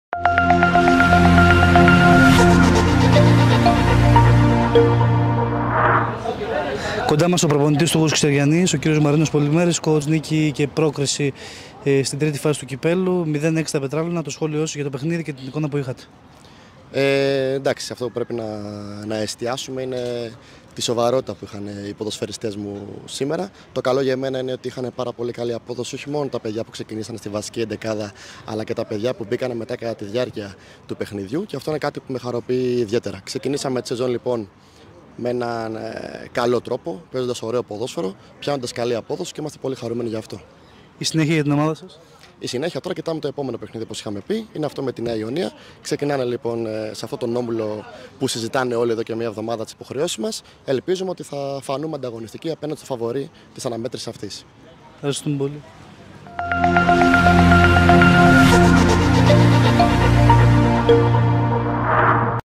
Πολυμέρης: Σημαντικό ότι δείξαμε σοβαρότητα στο πρώτο επίσημο ματς [vid]
Ο προπονητής του ΓΣ Καισαριανής, Μαρίνος Πολυμέρης, μίλησε για την πρόκριση επι των Πετραλώνων για το Κύπελλο ΕΠΣΑ